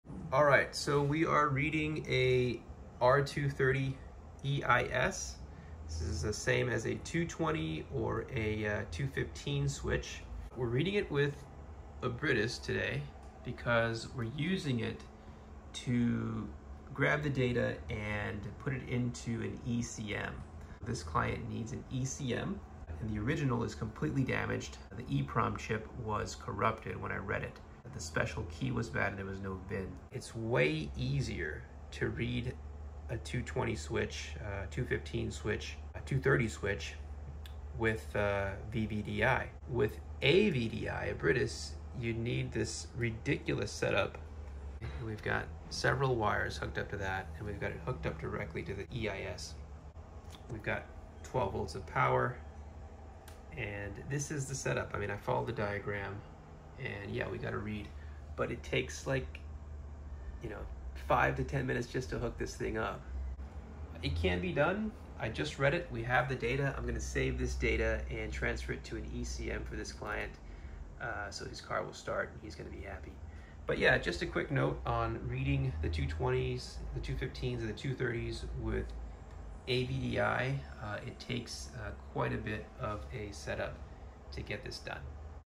Reading a Mercedes W220 W215 W230 EIS with Abrites AVDI.
What it takes to read a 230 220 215 EIS with Abrites AVDI.